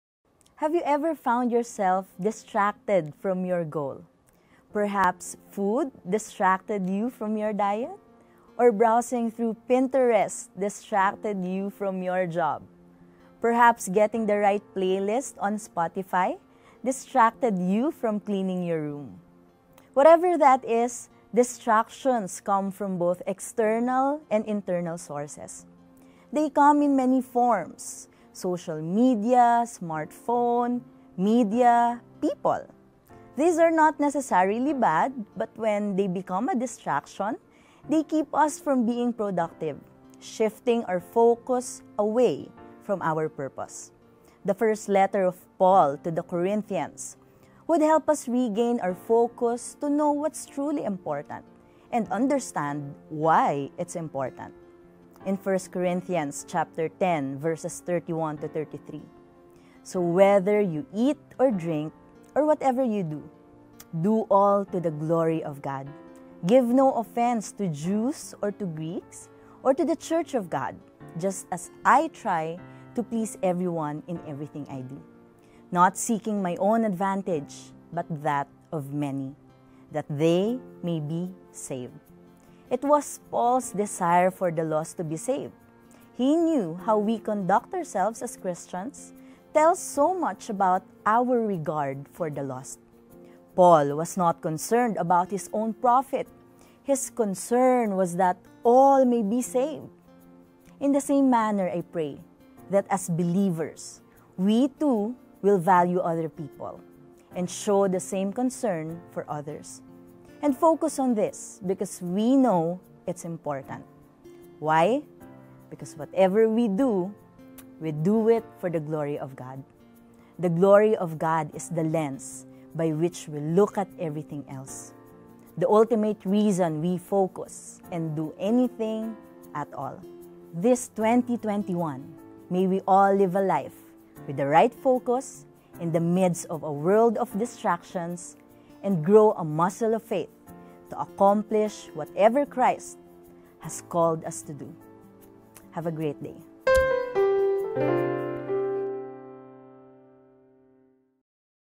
Focus — Daily Devo
1 Corinthians 10:31-33
Have you ever found yourself distracted from your goal? Coach Lenie helps us understand how the right desire — to do everything for God's glory — allows us to focus on what's truly important as we face the year ahead.

GET UPDATED

Listen to our podcasts on Spotify, Apple or Google Podcasts:
Victory Alabang Podcast